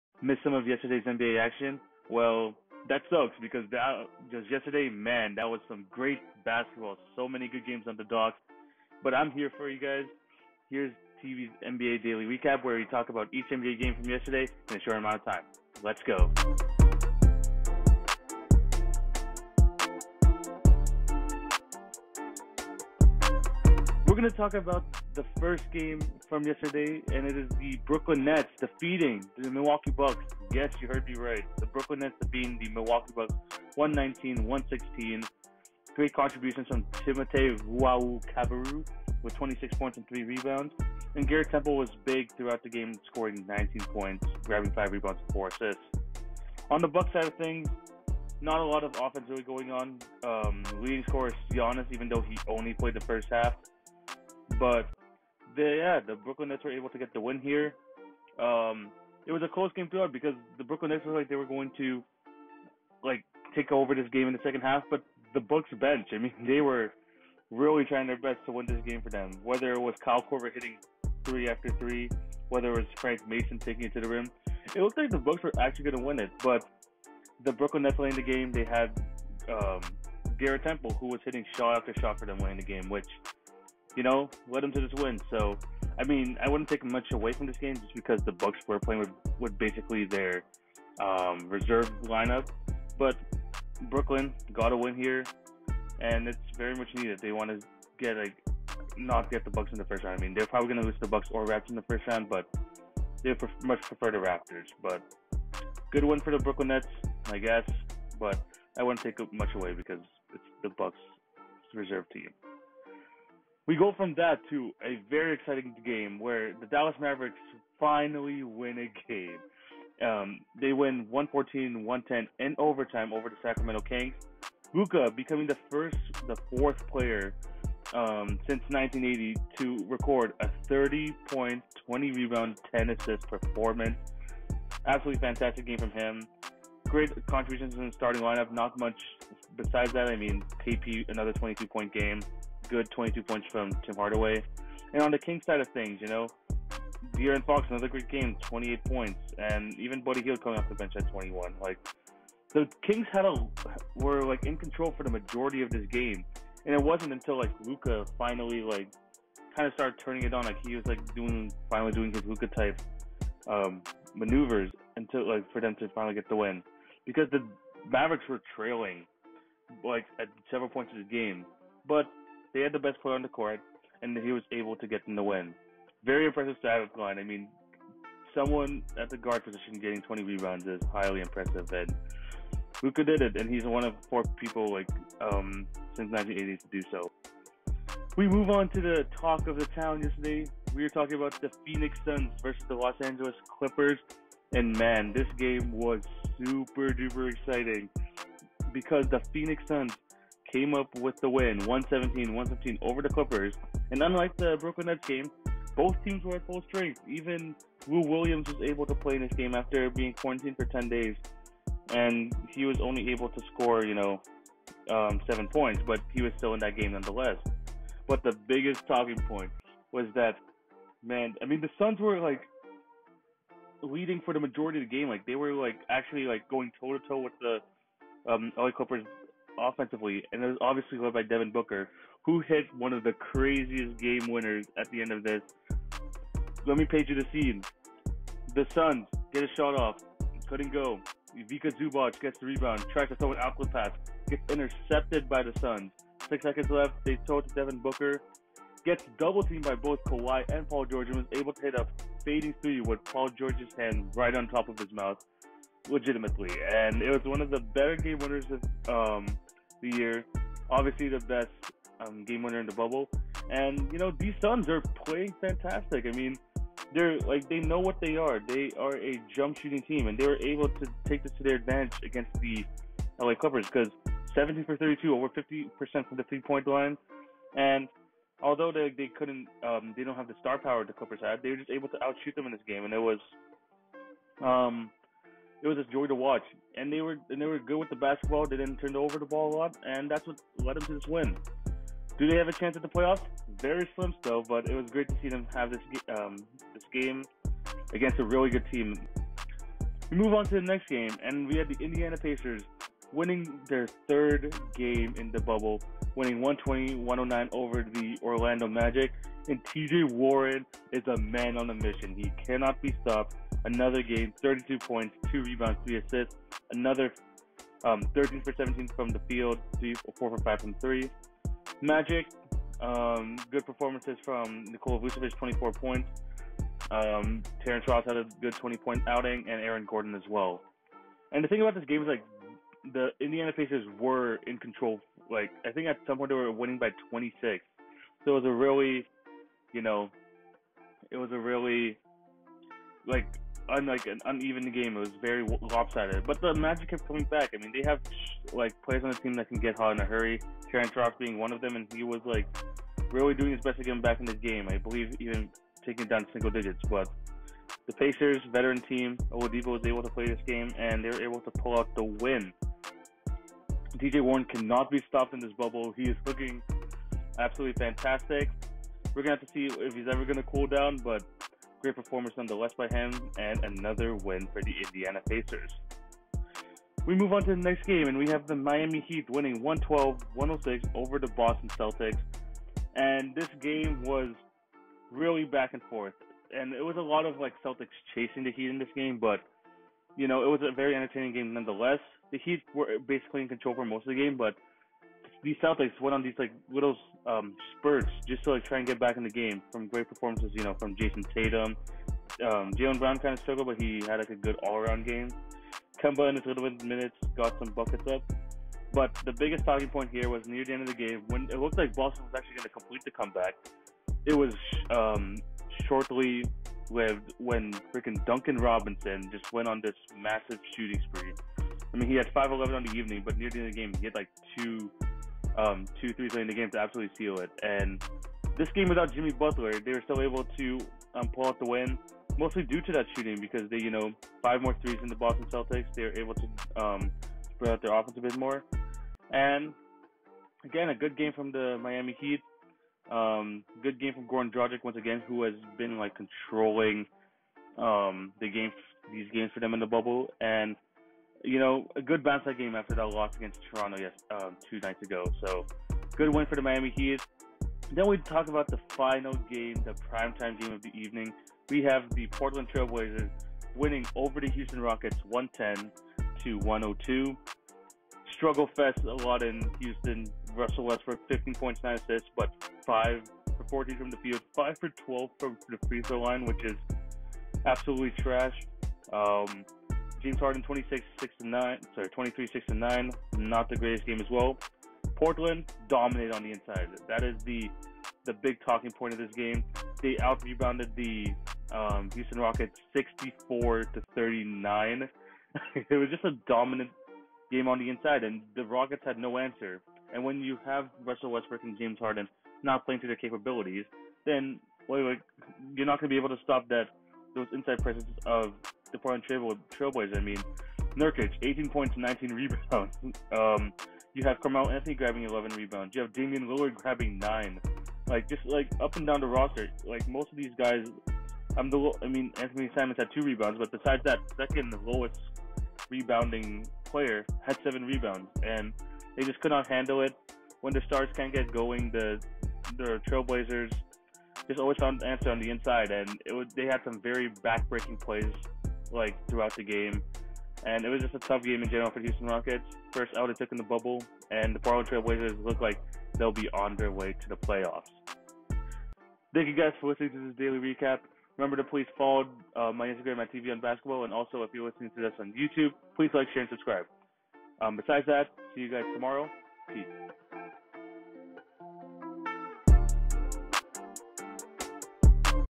TV's Daily NBA Recap: August 4 Games!
In today's recap, I discuss Booker's game-winner, Warren's continued hot streak, and more!

If you are looking for the best NBA recaps, NBA discussions, and more, look no further than TVonBasketball! Posting daily recaps and multiple discussions episodes a week!